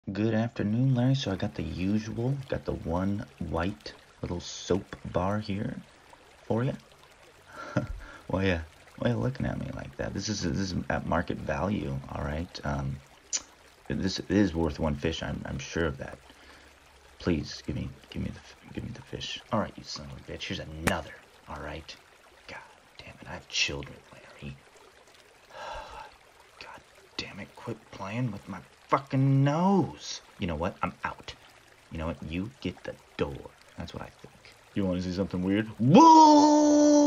Inspector Otter - Ep. 3
Otter tried to make a deal with Larry.